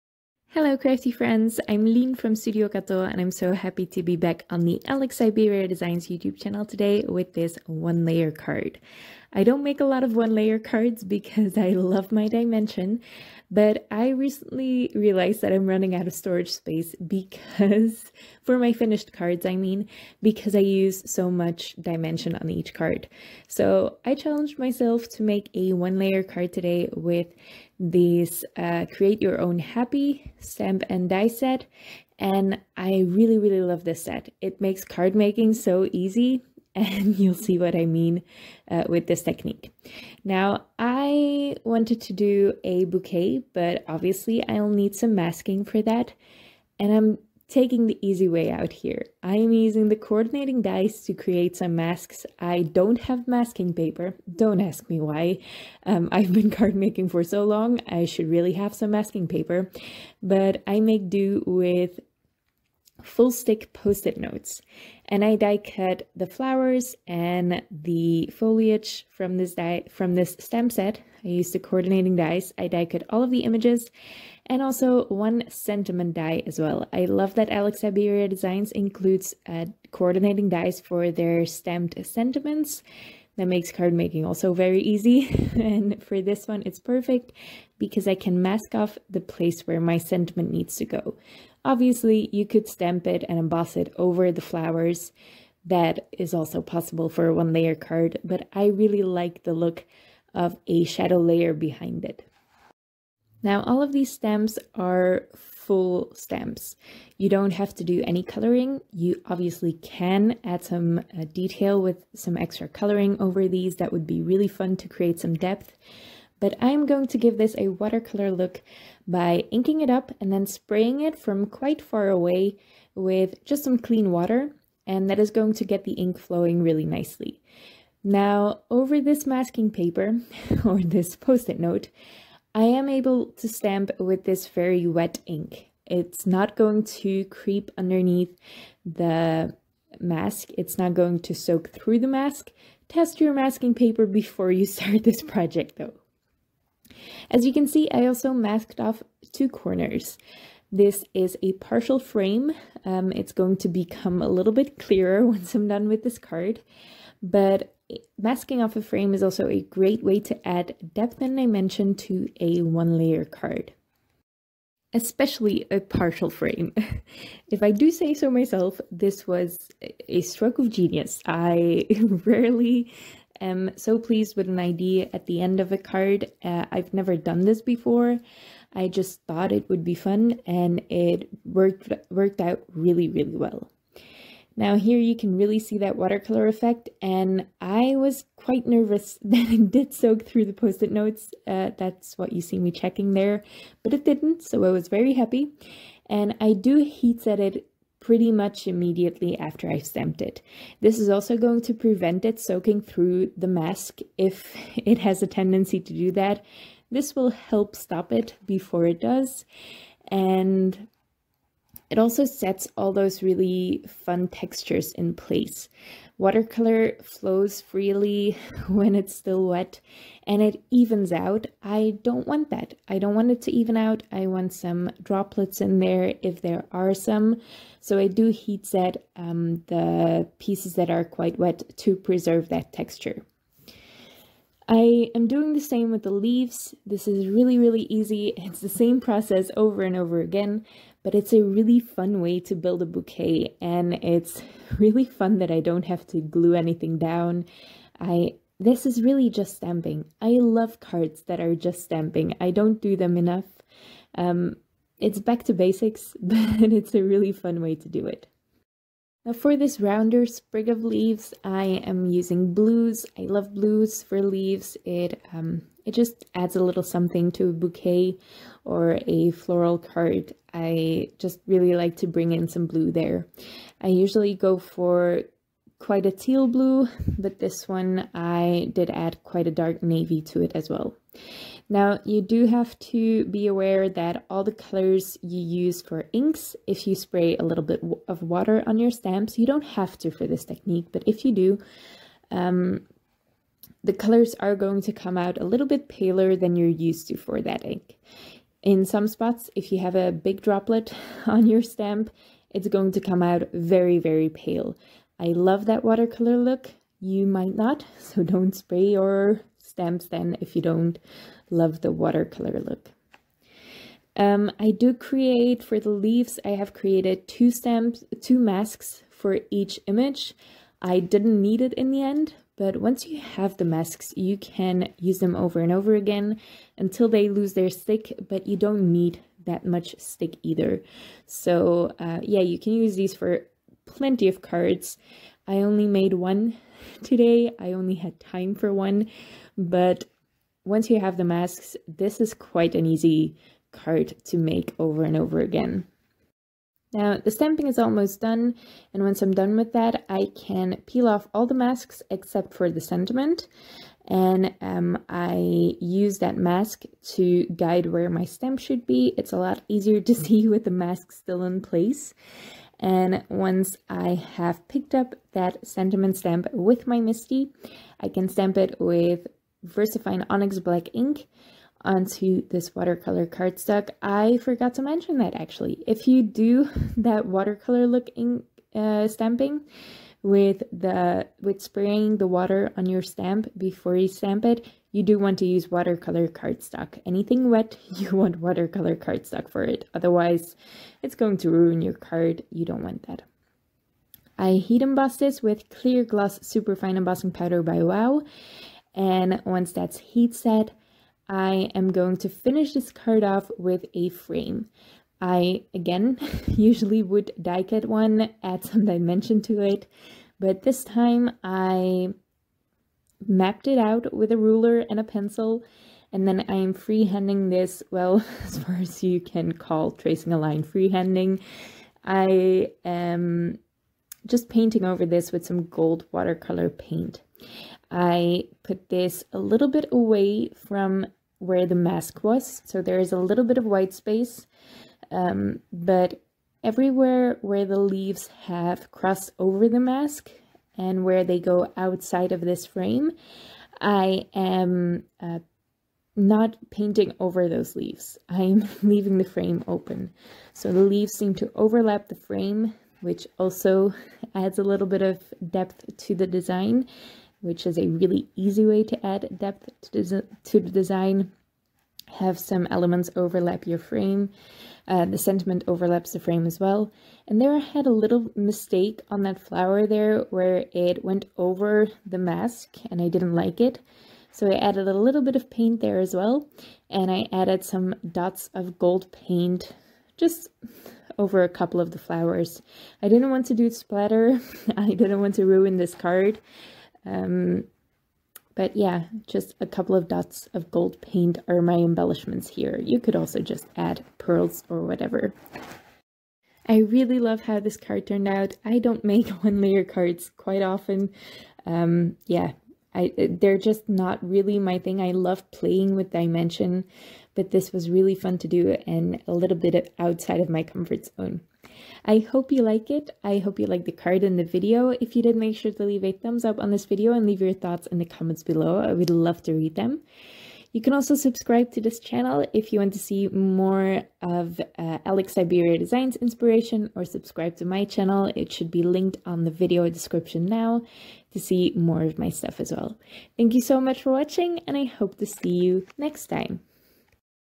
One Layer Wonder | Watercolor Stamping With Easy Masking
Hi crafty friends! I'm Lien from  @StudioKato back with a simple one layer wonder.

SUPPLIES
• Alex Syberia Designs - Create Your Own Happy stamp set

• Alex Syberia Designs - Create Your Own Happy die set

For some of my past cards and tutorials, make sure to visit my instagram and blog!